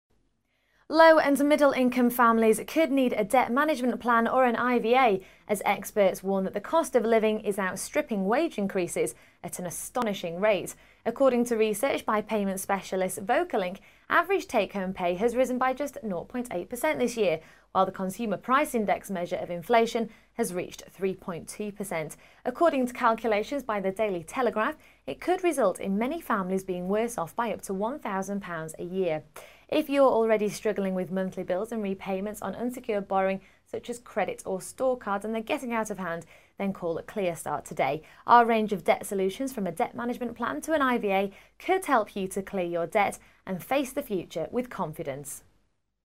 IVA as inflation outstrips wages
The cost of living is outstripping wages at a rapid rate, say experts, which could force many to turn to debt solutions like an IVA or DMP.